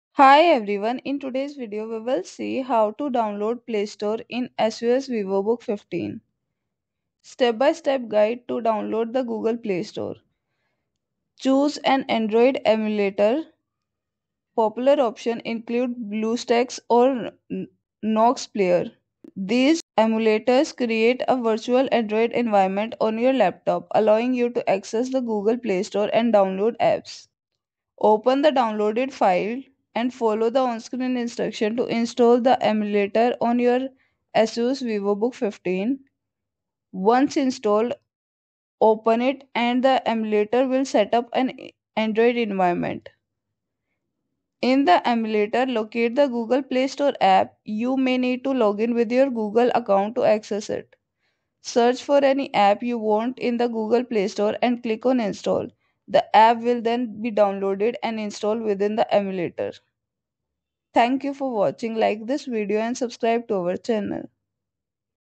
How To Download Playstore In ASUS VivoBook 15
In This Informative Video, We Will Guide You Through The Process Of Downloading The Google Play Store On Your Asus Vivobook 15. Whether You Are A New User Or Looking To Enhance Your Device's Functionality, Our Step-By-Step Instructions Will Ensure A Smooth Installation.

00:00 Intro
00:08 Download The Emulator
00:29 Install The Emulator
00:46 Access Google Play Store
00:51 Download Apps